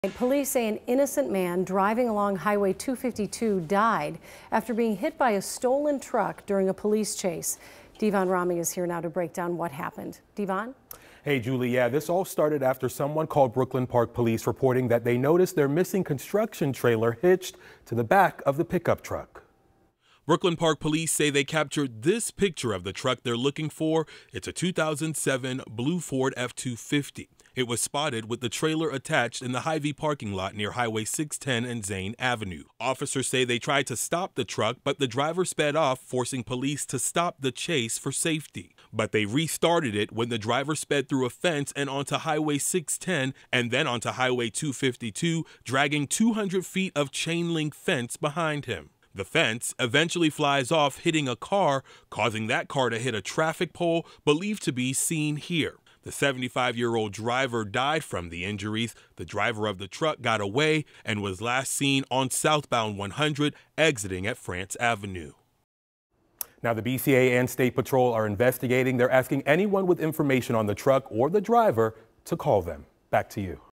Innocent motorist killed in crash involving alleged stolen vehicle, driver at-large
Police are looking for a truck that was involved in a fatal crash Tuesday morning in Brooklyn Park while fleeing police.